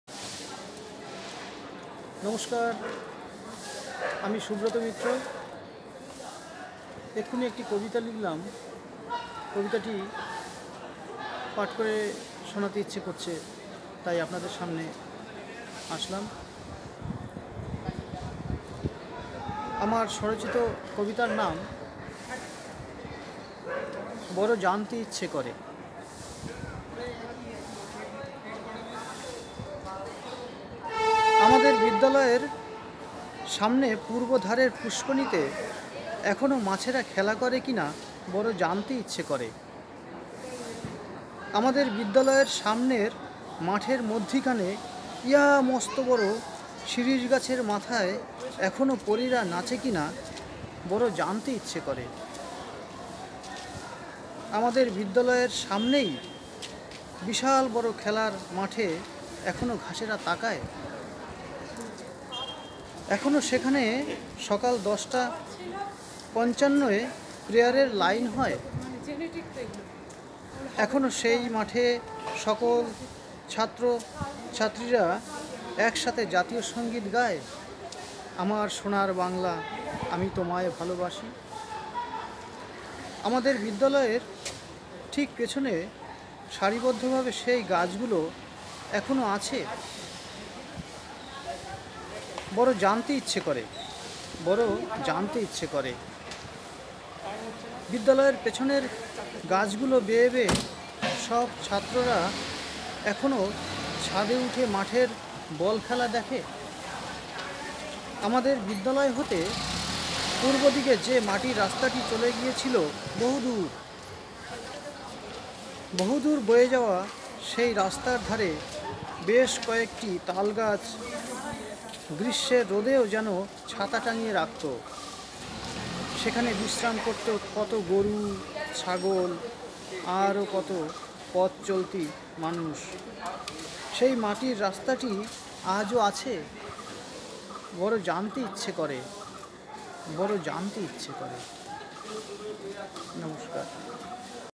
স্মৃতির গোচরে কবি সুব্রত মিত্র।স্বরচিত কবিতা-"বড় জানতে ইচ্ছে করে"।
বড় জানতে ইচ্ছে করে
সুব্রত মিত্র

আমাদের বিদ্যালয়ের সামনে---
পূর্ব ধারের পুষ্কনিতে এখনো মাছেরা খেলা করে কিনা

আমাদের বিদ্যালয়ের সামনে---
মাঠের মধ্যিখানে ইয়া মস্ত বড় শিরিষ গাছের মাথায়--
এখনও পরীরা নাচে কিনা

আমাদের বিদ্যালয়ের ঠিক সামনেই---
বিশাল বড় খেলার মাঠে এখনো ঘাসেরা তাকায়?
এখনো সেখানে সকাল দশটা পঞ্চান্নয় প্রেয়ারের লাইন হয়?
এখনো সেই মাঠে সকল ছাত্র-ছাত্রীরা একসাথে জাতীয় সংগীত গায়---

আমাদের বিদ্যালয়ের ঠিক পেছনেই---
সারিবদ্ধ ভাবে সেই গাছ গুলো এখনো আছে?
বিদ্যালয়ের পেছনের গাছগুলো বেয়ে বেয়ে সব--
ছাত্ররা এখনো ছাদে উঠে মাঠের বল খেলা দেখে?

আমাদের বিদ্যালয় হতে পূর্বদিকে মাটির রাস্তাটি চলে গিয়েছিল বহুদূর,,
বহুদূর বয়ে যাওয়া সেই রাস্তার ধারে---
বেশ কয়েকটি তালগাছ গ্রীষ্মের রোদেও যেন ছাতা টাঙ্গিয়ে রাখত,

সেখানে বিশ্রাম করত কত গরু; ছাগল; আরও কত পথ চলতি মানুষ,
সেই মাটির রাস্তাটি আজও আছে?